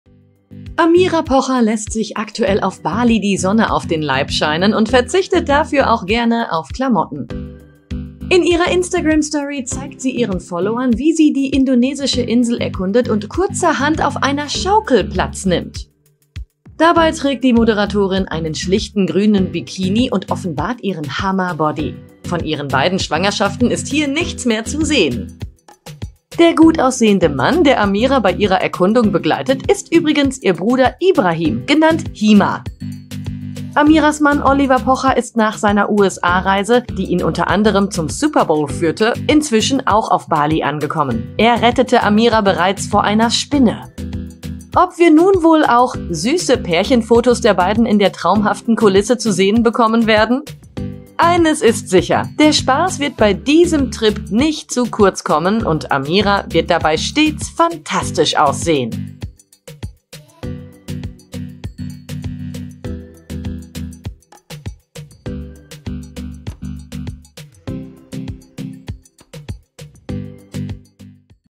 Hammer Body: Amira Pocher begeistert im Bikini | It's in TV
Amira Pocher lässt sich aktuell auf Bali die Sonne auf den Leib scheinen und verzichtet dafür auch gerne auf Klamotten.